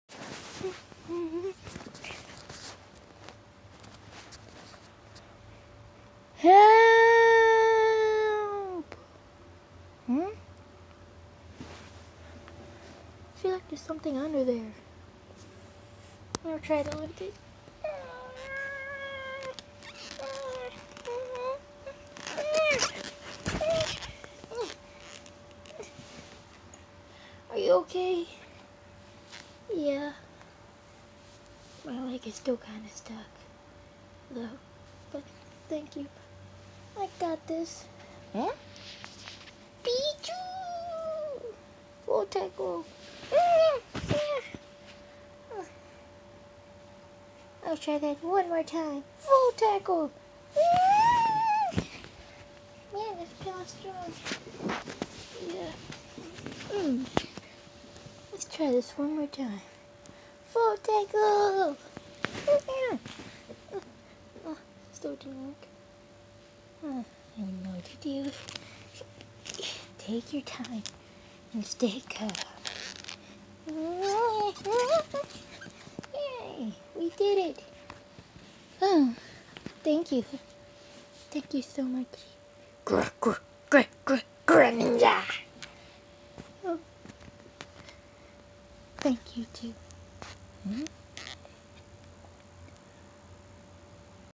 Greninja and the cave of ducks part 1
Primarina finds somebody under a giant pillow and saves them with pichus help it is a pokemon called greninja.He says that he has heard that psyduck is somewhere hidden in the cave of ducks.